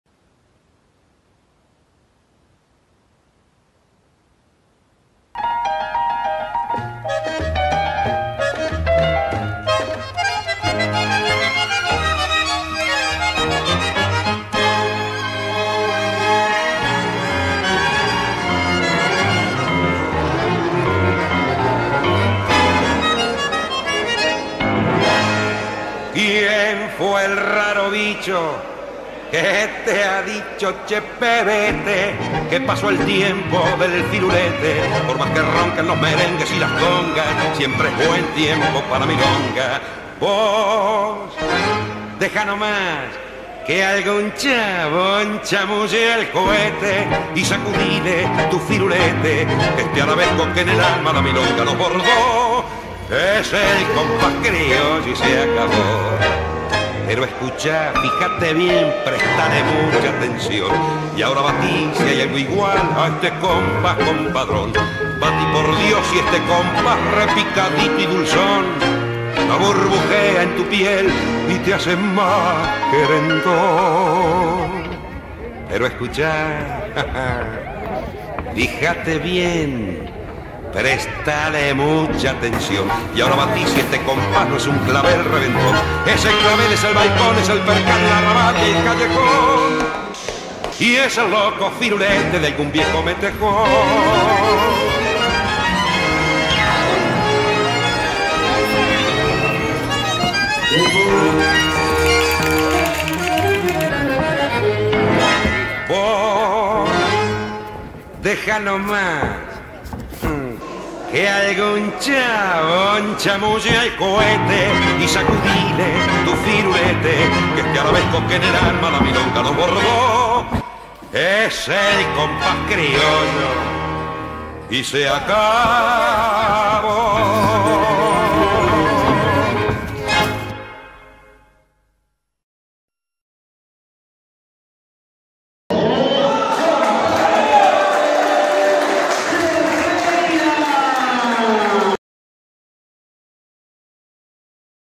boxeo
El firulete y algunos videos que tenia de peleas viejas jajaj